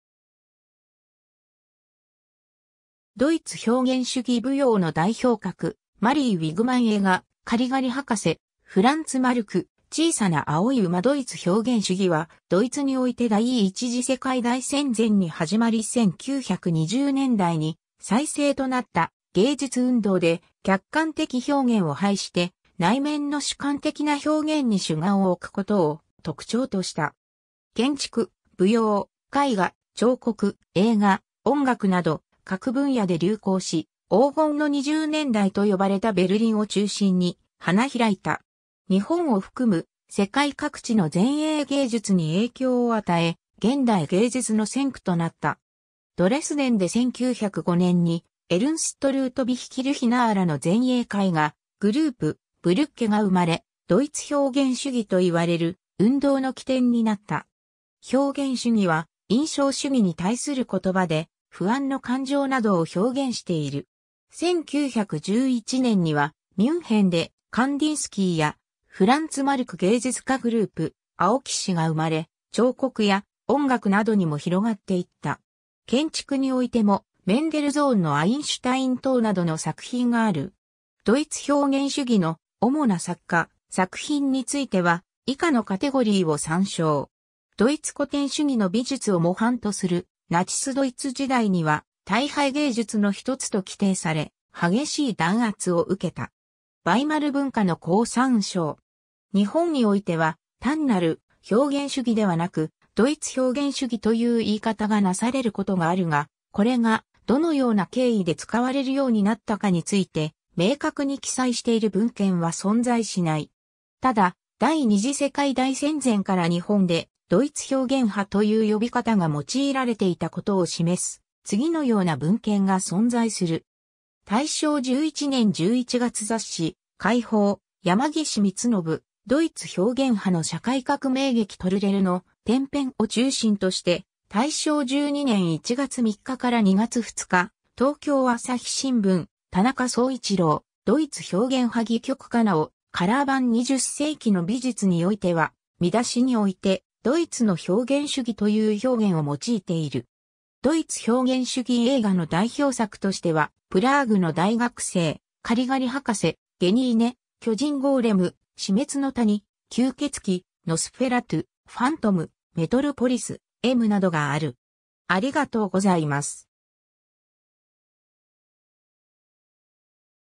ドイツ表現主義
ドイツ表現主義舞踊の代表格マリー・ウィグマン 映画「カリガリ博士」 フランツ・マルク「小さな青い馬」 ドイツ表現主義（英: German Expressionism）は、ドイツにおいて第一次世界大戦前に始まり1920年代に最盛となった芸術運動で、客観的表現を排して内面の主観的な表現に主眼をおくことを特徴とした。
建築、舞踊、絵画、彫刻、映画、音楽など各分野で流行し、「黄金の20年代」と呼ばれたベルリンを中心に花開いた。
日本を含む世界各地の前衛芸術に影響を与え、現代芸術の先駆となった。
 ドレスデン（ドイツ帝国時代のザクセン王国）で1905年に、エルンスト・ルートヴィヒ・キルヒナーらの前衛絵画グループ「ブリュッケ」が生まれ、ドイツ表現主義と言われる運動の起点になった（表現主義の項目を参照のこと）。
表現主義（Expressionism）は印象主義（Impressionism）に対する言葉で、不安の感情などを表現している。
1911年にはミュンヘンでカンディンスキーやフランツ・マルク芸術家グループ「青騎士」が生まれ、彫刻や音楽などにも広がっていった。
建築においても、メンデルゾーンのアインシュタイン塔などの作品がある。
ドイツ表現主義の主な作家・作品については以下のカテゴリーを参照。
 ドイツ古典主義の美術を模範とするナチス・ドイツ時代には、「退廃芸術」の一つと規定され、激しい弾圧を受けた。
ヴァイマル文化の項参照。
 日本においては、単なる「表現主義」（「表現派」）ではなく「ドイツ表現主義」（「ドイツ表現派」）という言い方がなされることがあるが、これがどのような経緯で使われるようになったかについて明確に記載している文献は存在しない。
ただ、 第二次世界大戦前から日本で「ドイツ表現派」という呼び方が用いられていたことを示す、次のような文献が存在する。
 大正11年（1922年）11月雑誌『解放』：山岸光宣「独逸表現派の社会革命劇　トルレルの『転変』を中心として」 大正12年（1923年）1月3日-2月2日『東京朝日新聞』：田中總一郎「独逸表現派戯曲家」 なお、『カラー版20世紀の美術』（監修・末永照和、美術出版社、2000年）においては、見出しにおいて（「ドイツ表現主義」という表現ではなく）「ドイツの表現主義」という表現を用いている（11ページ）。
 ドイツ表現主義映画の代表作としては『プラーグの大学生』、『カリガリ博士』、『ゲニーネ』、『巨人ゴーレム』、『死滅の谷』、『吸血鬼ノスフェラトゥ』、『ファントム』、『メトロポリス』、『M』などがある。